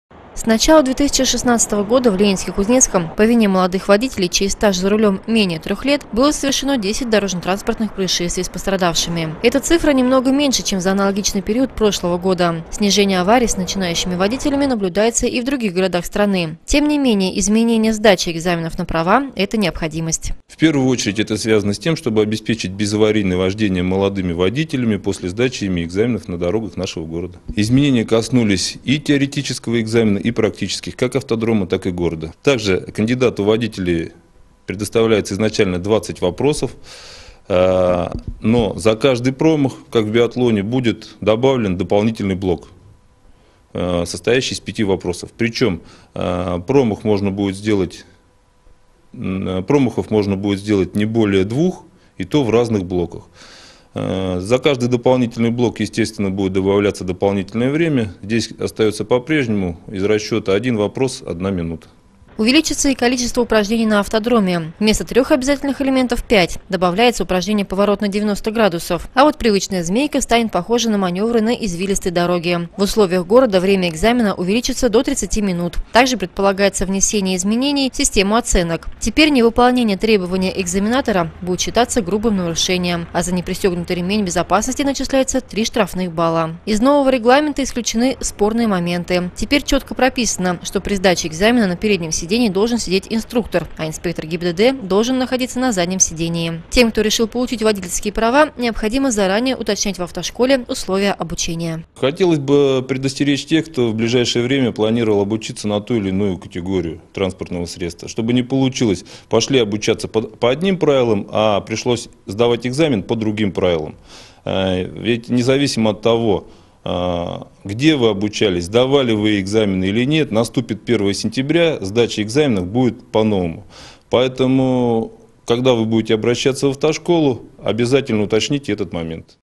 С 1 сентября усложнится экзамен на получение водительских прав
Для будущих водителей изменятся правила сдачи экзаменов на права. Основные положения были утверждены правительством еще в 2014 году, и за это время проект неоднократно дорабатывался. МВД учло предложения и замечания  автошкол и граждан,  и представило новый регламент, который вступит в силу с 1 сентября этого года. Нововведения касаются как теоретической, так и практической части экзамена.